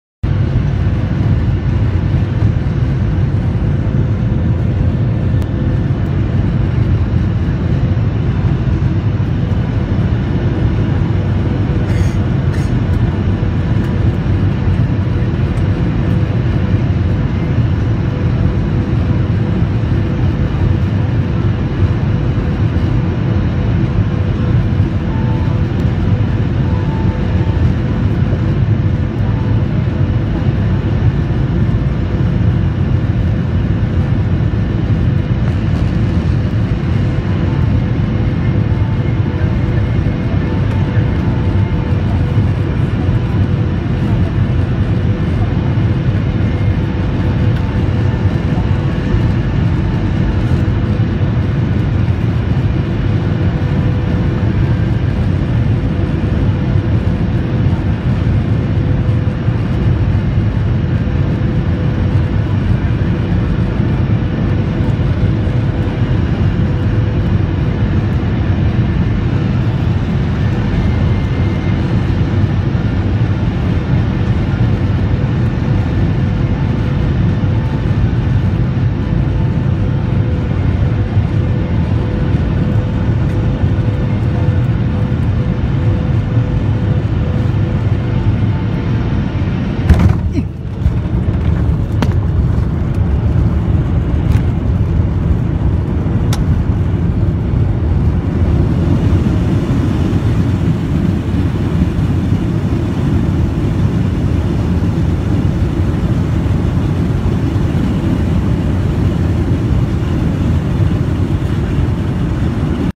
Jet Airways Boeing 737 800 Hard Landing
Jet Airways hard landing during a flight.